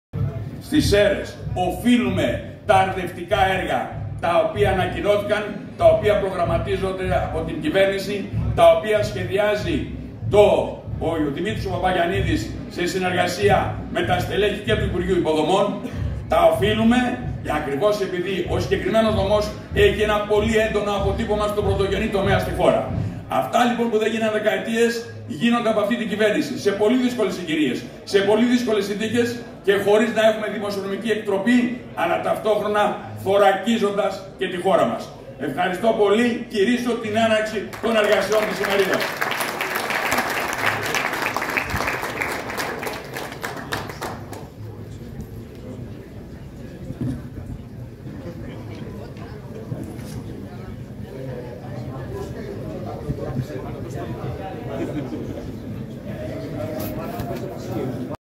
Ο Υπουργός Αγροτικής Ανάπτυξης & Τροφίμων Γ. Γεωργαντάς στις Σέρρες (24/09/2022)
✎ Διαβάστε το σχετικό ρεπορτάζ του Infonews24 ☛

► Η Ελλάδα έχει τη δυνατότητα να κερδίσει το στοίχημα της προσαρμογής της παραγωγής της στο στόχο της ΕΕ για βιολογική παραγωγή, καθώς η πολιτική ηγεσία του ΥπΑΑΤ δημιουργεί τις προϋποθέσεις γι’ αυτό, τόνισε ο υπουργός Αγροτικής Ανάπτυξης και Τροφίμων κ. Γιώργος Γεωργαντάς, σε ομιλία του στην ημερίδα για τα βιολογικά προϊόντα που διοργάνωσε η Γ.Γ. Ενωσιακών Πόρων και Υποδομών σε συνεργασία με το Επιμελητήριο Σερρών, στο πλαίσιο των εργασιών της SerExpo.